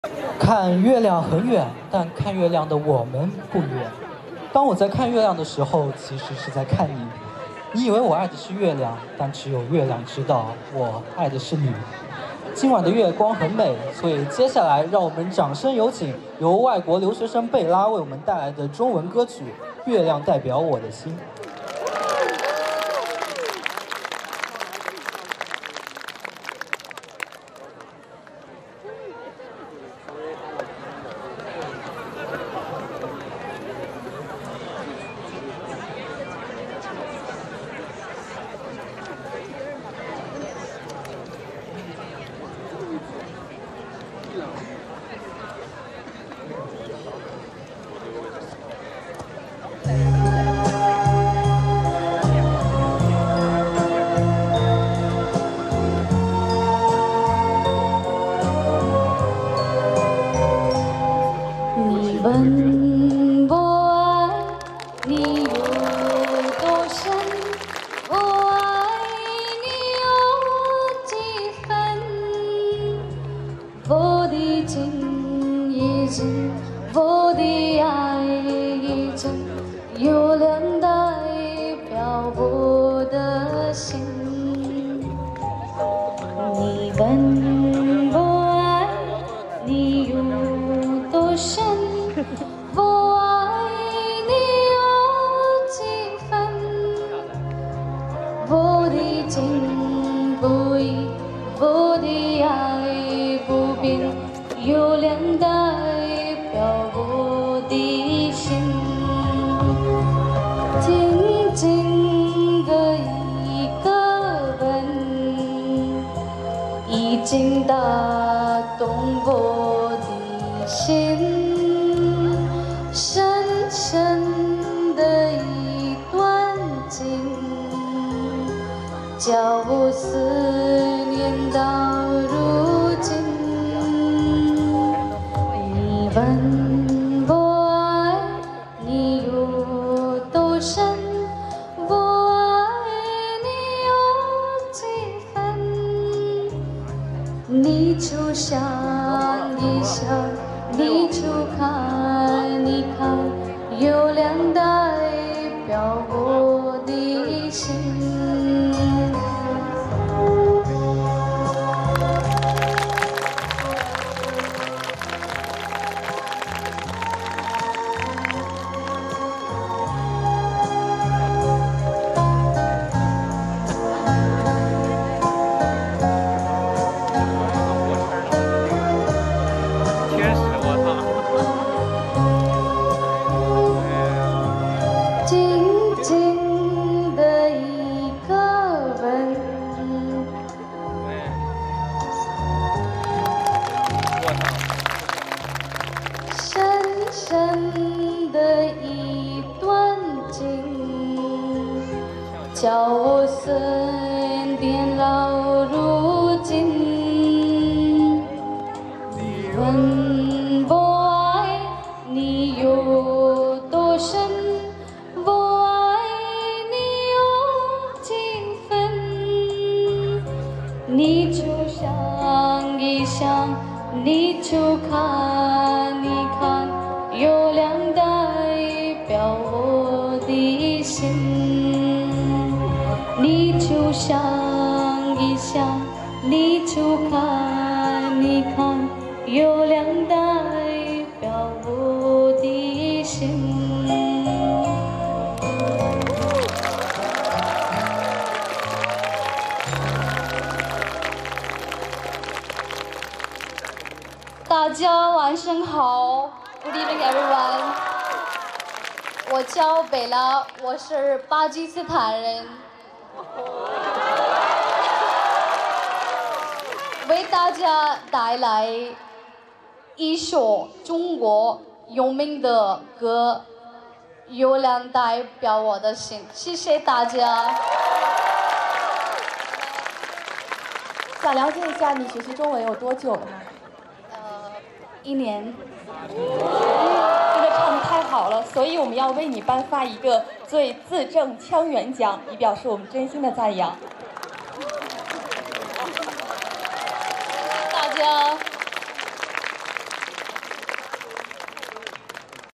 Famous Chinese Song | Performed | University concert | 2023 🇨🇳🇵🇰
Performed Famous Chinese song for the first time at Ningbo, China University